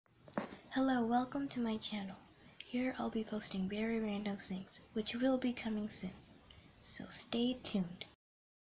Welcome! (Read description)
Hello, hi, and welcome! Like I said I'll be posting random things. My next video will most likely be about a mini piano tutorial, a gameplay, or some stray cats!
 (っ.❛ ᴗ ❛.)っ